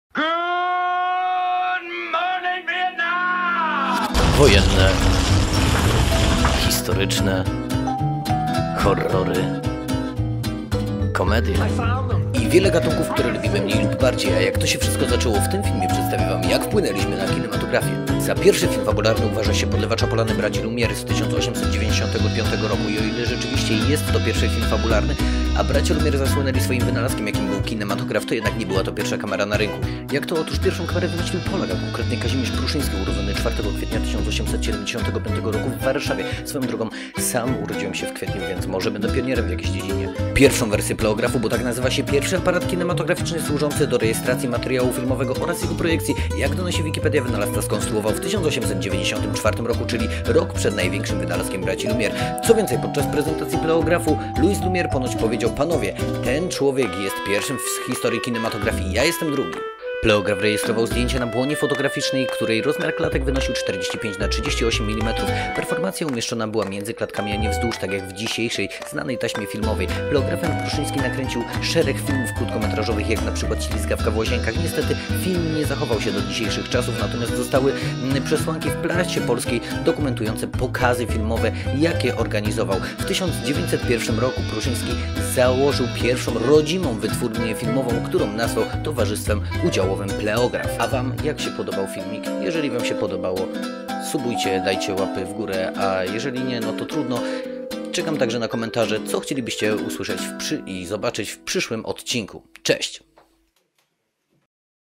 Polak przed braćmi Lumiere
Polak przed braćmi Lumiere to któtki film pokazujący jaki mieliśmy wpływ na kinematografię na świecie. Przedstawia któtką historię Kazimierza Prószyńskiego człowieka który wymyślił pierwsze urządzenie do nagrywania i projekcji filmów jakim był Pleograf

Programy i urzyta technologia:
Canon 7D/ Tamaron 28-75mm F2.8
Behringer XENYX Q502USB
Behringer ULTRA GRAPH PRO GEQ3102
Samson C01 Studio Condenser
Color Grading: Davinci Resolve 12.5
Edycja: Adobe Premiere Pro CC
Muzyka: Adobe Audition CC